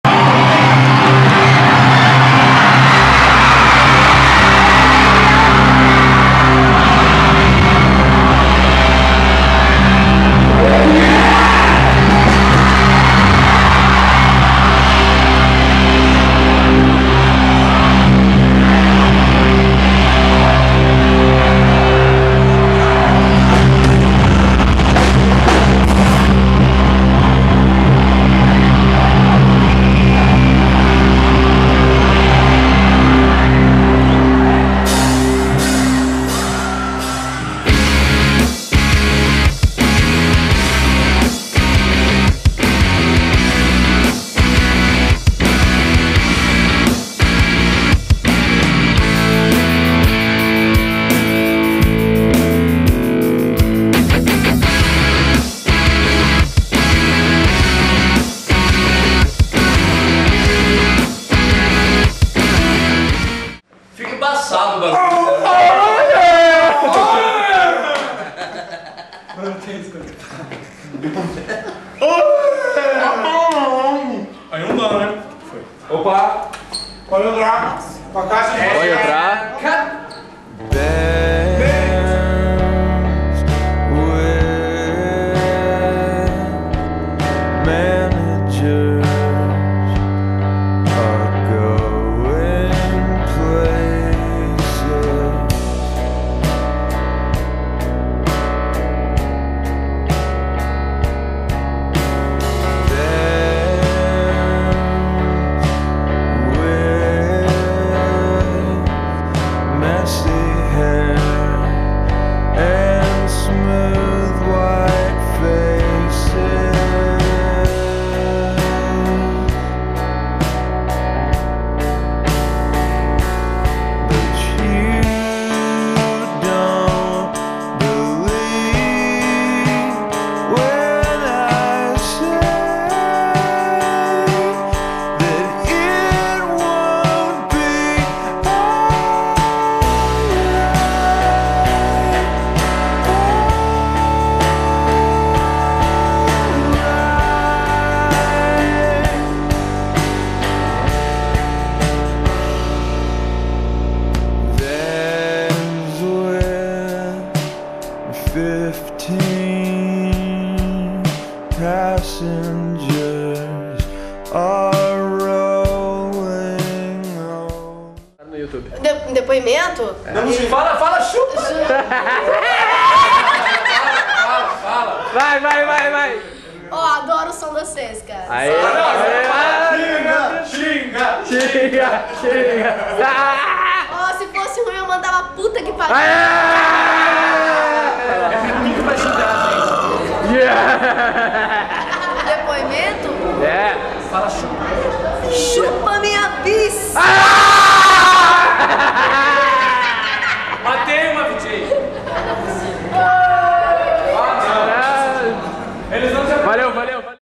fresno@são paulo with yellowcard
* Cenas dos bastidores do show na Via Funchal com os americanos do Yellowcard. Ultra FODA!

* Trilha sonora: Pedro The Lion